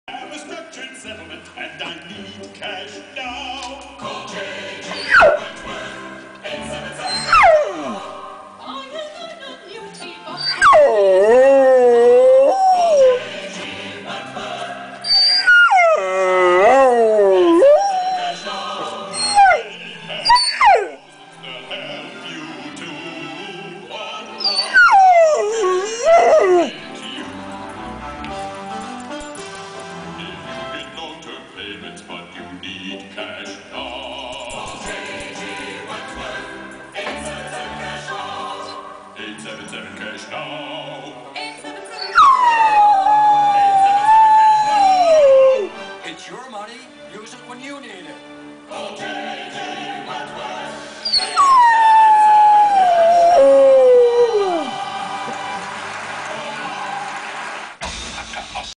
pit bull sings to commercial
pipsqueak, a pit bull sings to a commercial